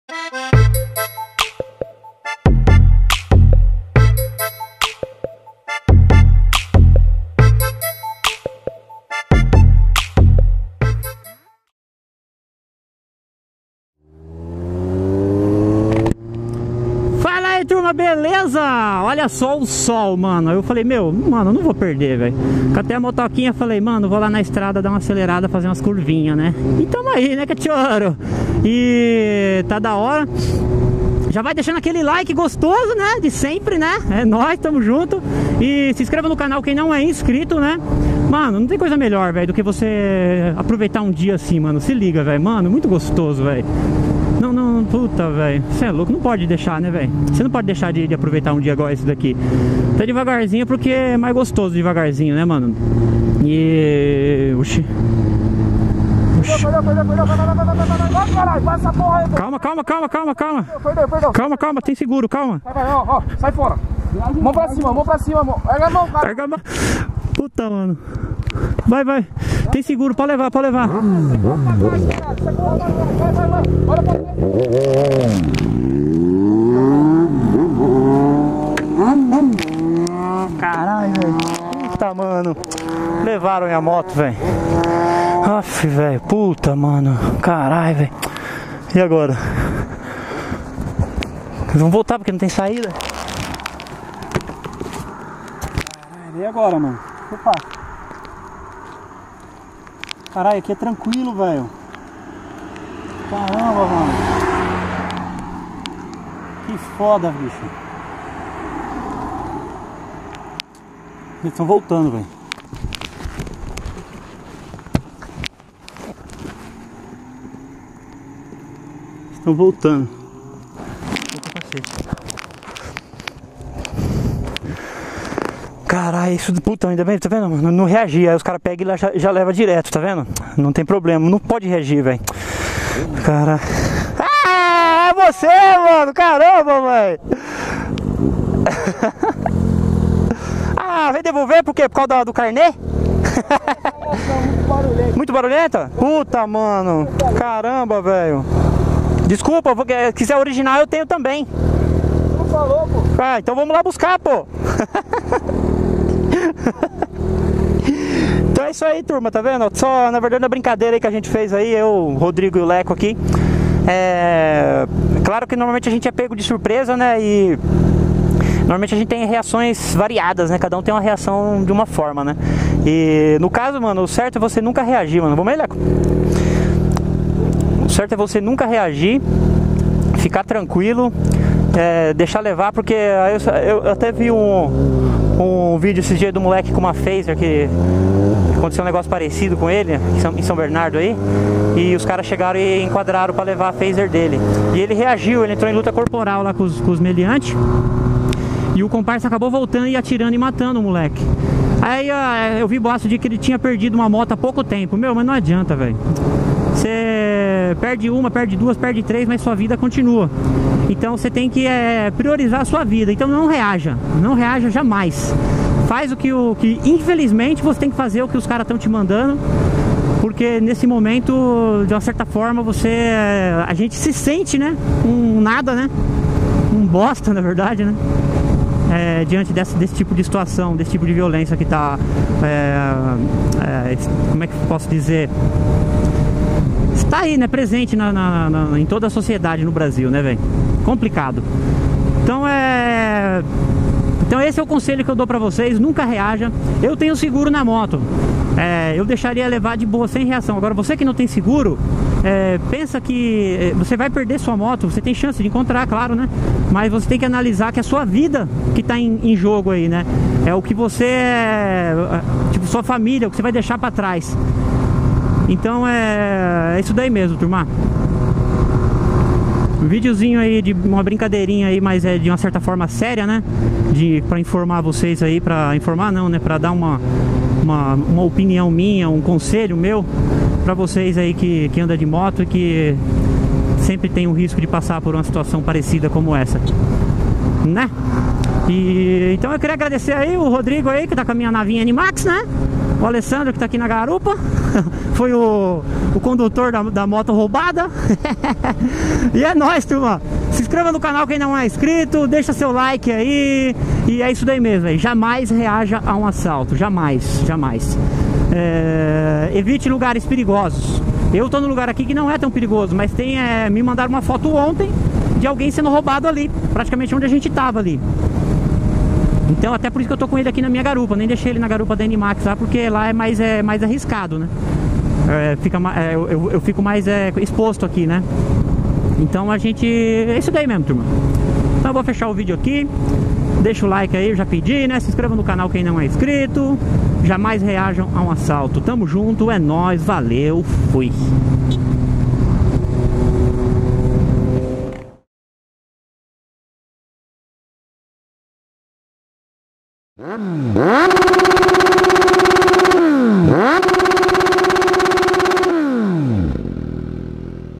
Roubaram minha moto, como eu reagiria! *ATA-GSR750*
FAAAALA AEEEE TURMA
ATENÇÃO
Se trata de um video feito para alertar as pessoas para que não reajam ao serem abordados por ladrões. infelizmente isso é uma realidade que temos que encarar, em um pais que não há investimentos em educação e cultura, vivemos a mercê de bandidos e assassinos. Enquanto não houver uma mudança cultural em nossa sociedade, vamos ter que nos preparar  para acontecimentos como este...
NUNCA, MAS NUNCA MESMO REAJAM EM UM ASSALTO.
✔️QUER ADQUIRIR UMA CAMISETA DO CANAL❓
🔶GOSTOU DO VIDEO❓
🔸ENTAO DEIXE SEU JOINHA👍🏼👍🏼
🔸atagsrsnap